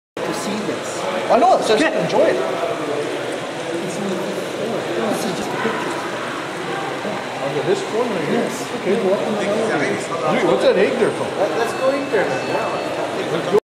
Cuba (35).Earnest Hemmingway's Hotel in Havana
Earnest Hemmingway's hotel in Havana, Cuba. Cuba, Havana, vacations, vacation, westjet, resorts, Sirenis La Salina,